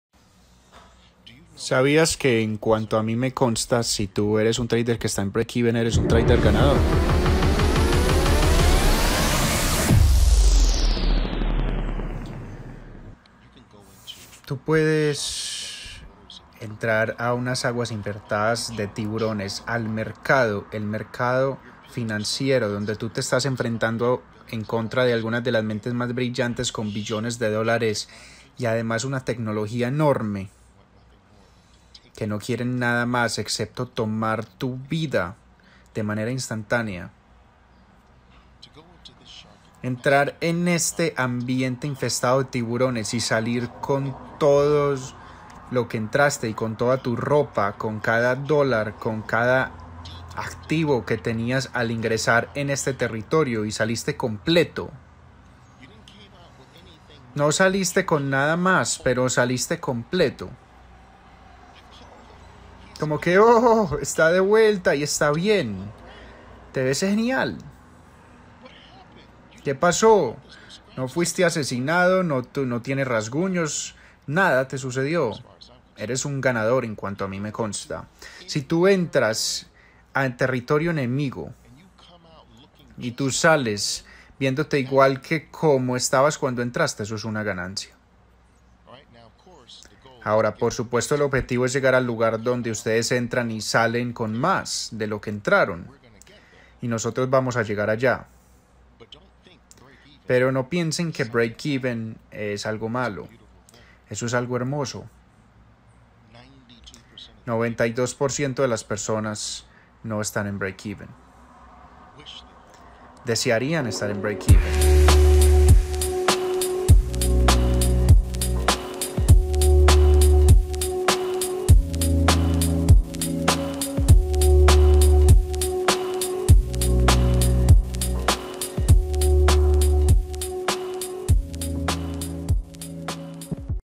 Breakeven es
Oliver Velez considera que la manera más rápida para llegar al éxito en trading es dejar a un lado el miedo de perder tu propio dinero. Es este miedo que muchas veces se convierte en un obstáculo para llegar al éxito. No solo aprenderás las 14 tácticas esenciales de Oliver, también serás totalmente financiado por el. Deja tu dinero a un lado. Opera el dinero de Oliver. ¡Escribe hoy!